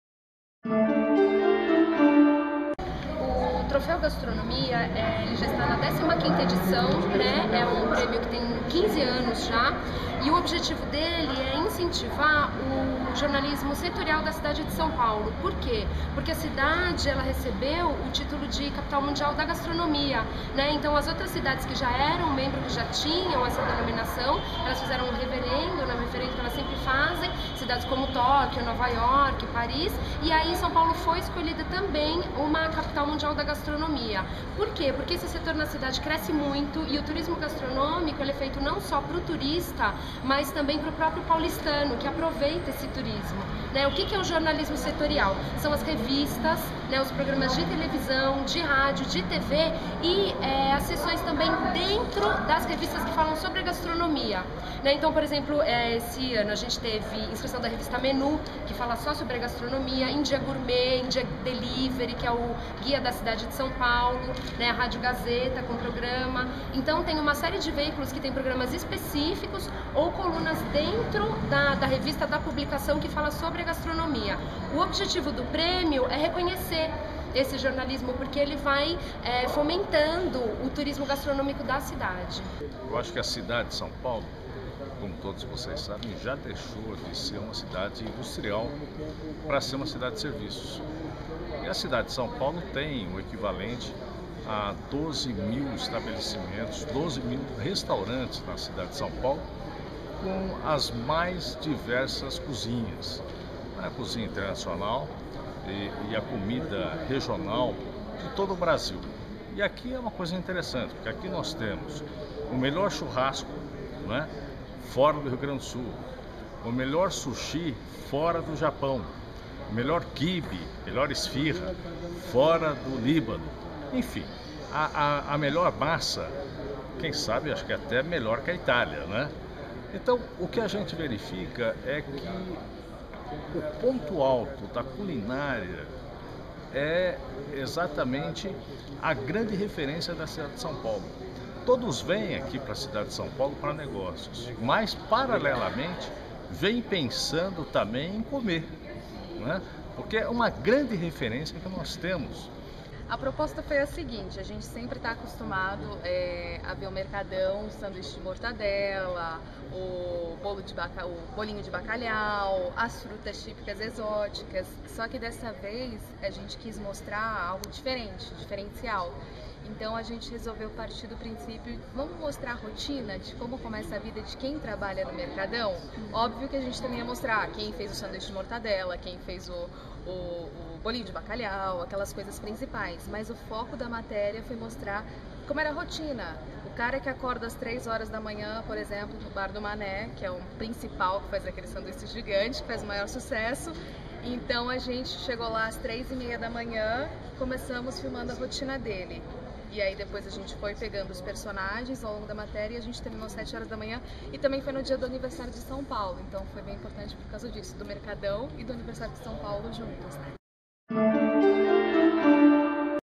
15º Troféu São Paulo Capital Mundial da Gastronomia
Sessão Solene de entrega do 15º Troféu São Paulo Capital Mundial da Gastronomia, instituído pelos Decretos Legislativos 81 de 1997 e 16 de 2002. Uma iniciativa da Comissão de Trânsito, Transporte, Atividade Econômica, Turismo, Lazer e Gastronomia da Câmara Municipal de São Paulo.